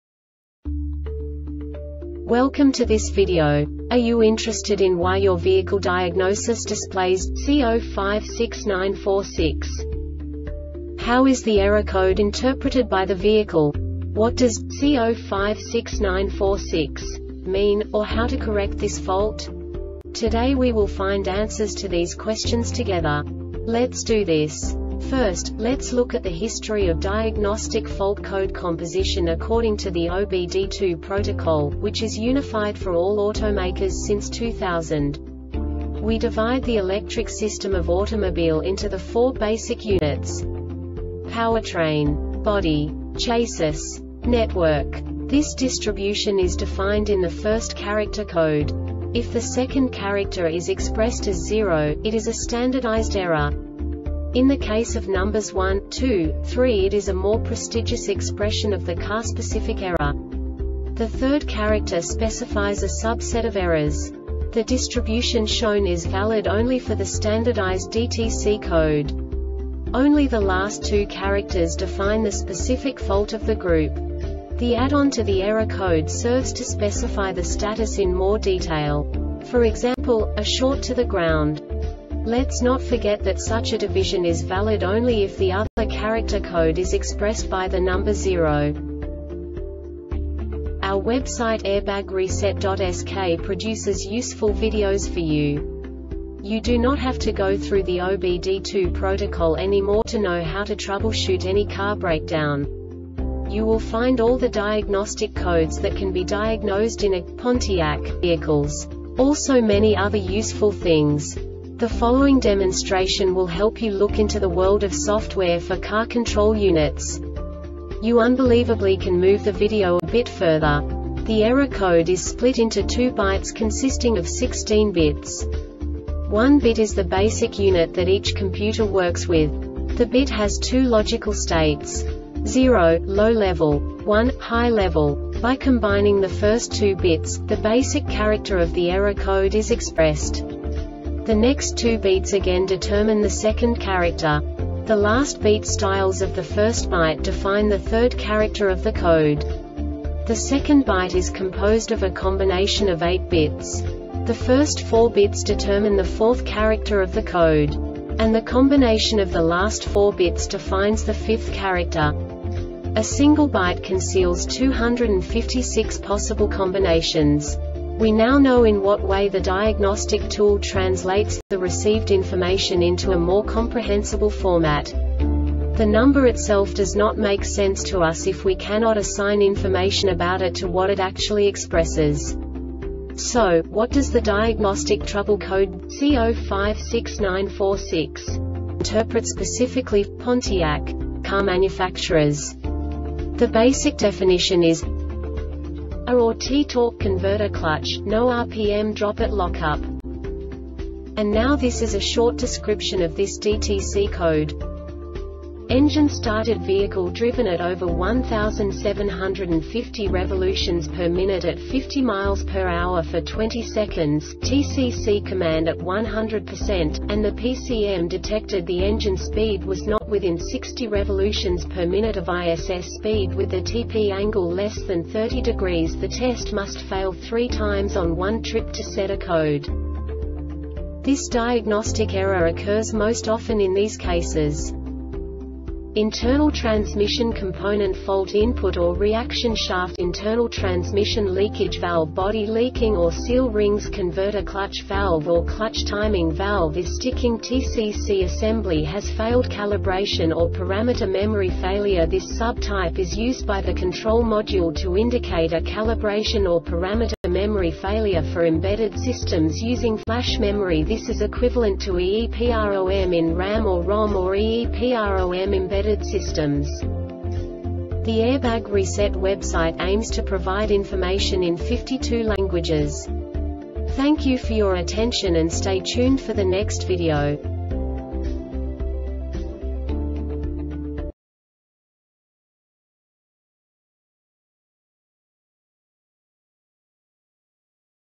DTC Pontiac C0569-46 Short Explanation
Contents:
0:21 Basic DTC analysis according to OBD2 protocol standard.
1:48 Insight into programming
2:58 A diagnostically understandable description of the Diagnostic Trouble Code
3:05 Basic error definition

Make:
Pontiac
Code:
C0569 46
Definition:
A or T Torque Converter Clutch - No RPM Drop At Lockup
Description:
Engine started;vehicle driven at over 1750 rpm at 50 mph for 20 seconds,TCC command at 100%,and the PCM detected the engine speed was not within 60 rpm of ISS speed with the TP angle less than 30 degrees.The test must fail 3 times on one trip to set a code.
Cause:
Internal transmission component fault (Input or Reaction shaft)Internal transmission leakage (valve body leaking or seal rings)Converter clutch valve or clutch timing valve is stickingTCC assembly has failed
Failure Type:
CalibrationorParameter Memory Failure
This sub type is used by the control module to indicate a calibrationorparameter memory failure for embedded systems using FLASH memory. This is equivalent to EEPROM in RAM or ROM or EEPROM embedded systems.